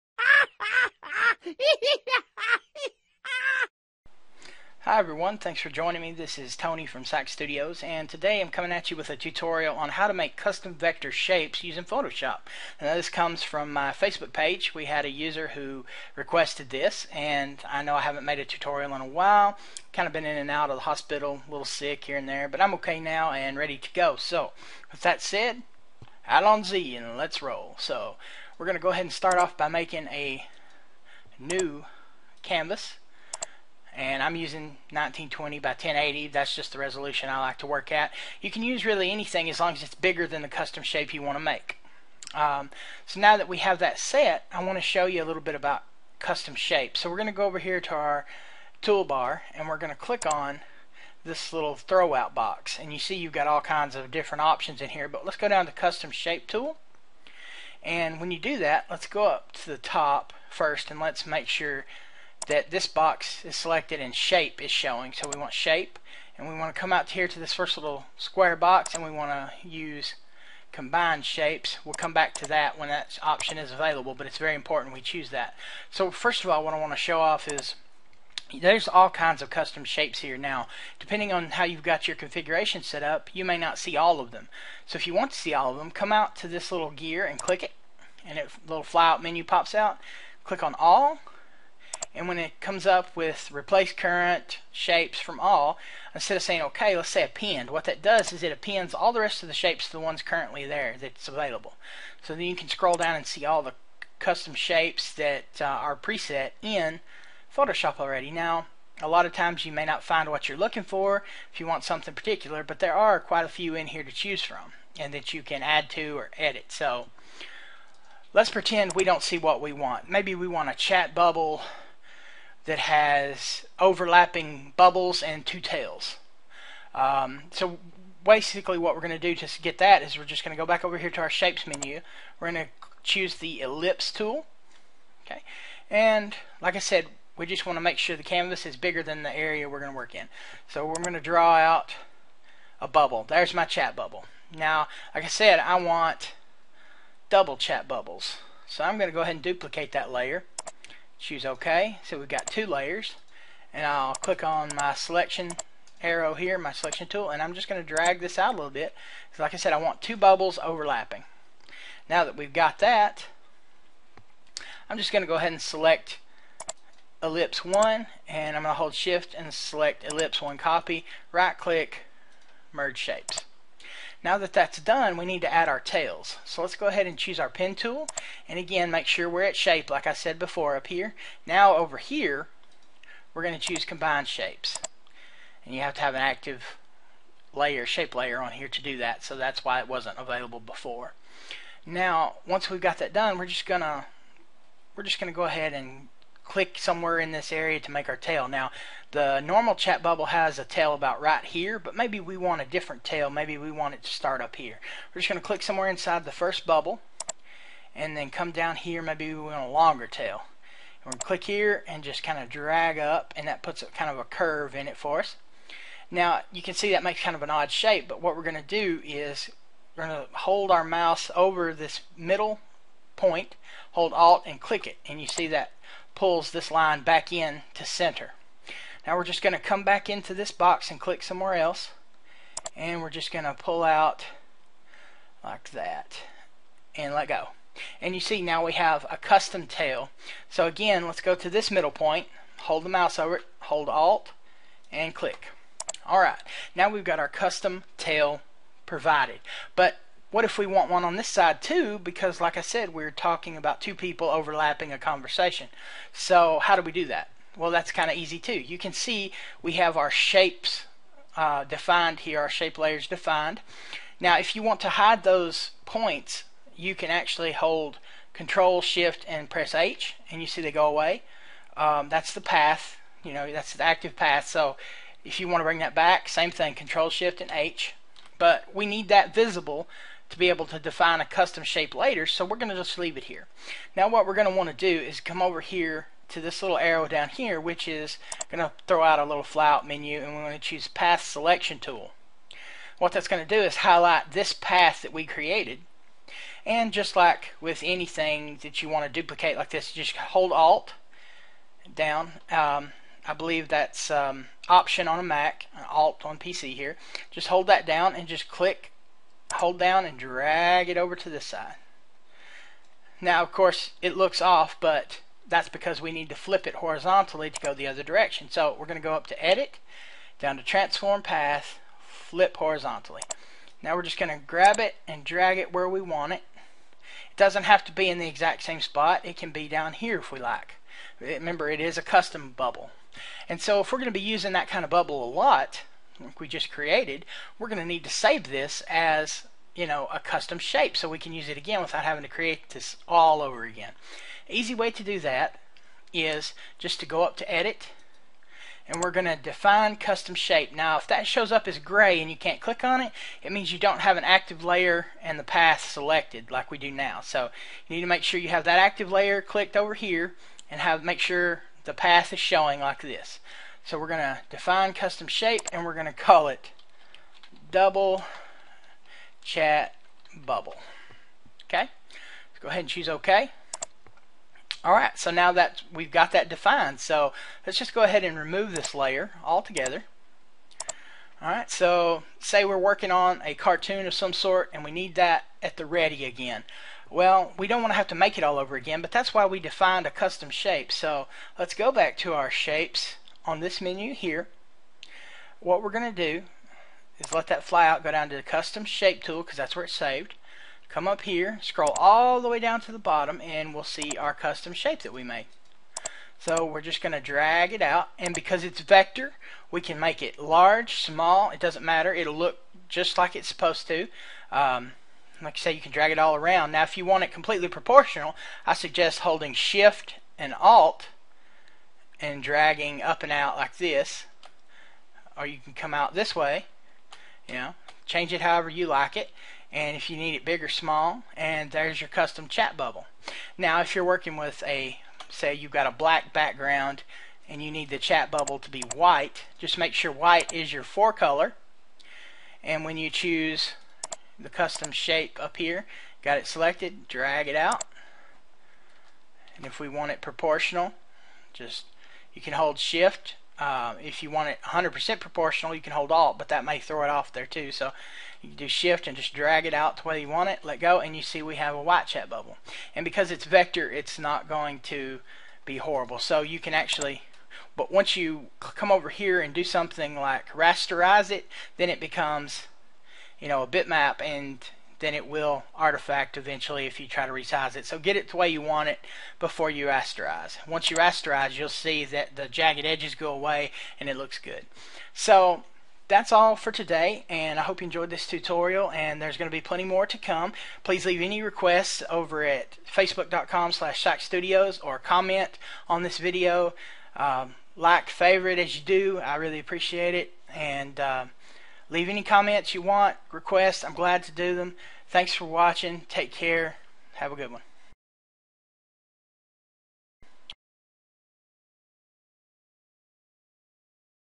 Creating Custom Vector Shapes - Photoshop Tutorial
In this tutorial, we tackle a Facebook fanpage request on how to create custom vector shapes. We apologize for not uploading a video for a while, but more regular videos are on the way. We hope you enjoy this tutorial. Thanks